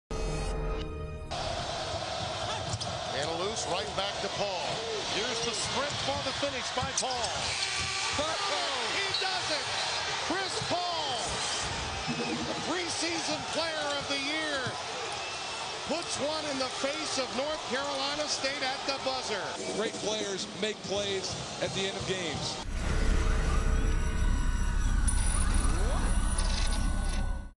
Chris Paul Buzzer Beater Game Winner | ACCDigitalNetwork
With Chris Paul's last second heroics in Monday's win against the Memphis Grizzlies in the NBA Playoffs, we take a look back at another game-winner Paul had at Wake Forest in 2005 against NC State.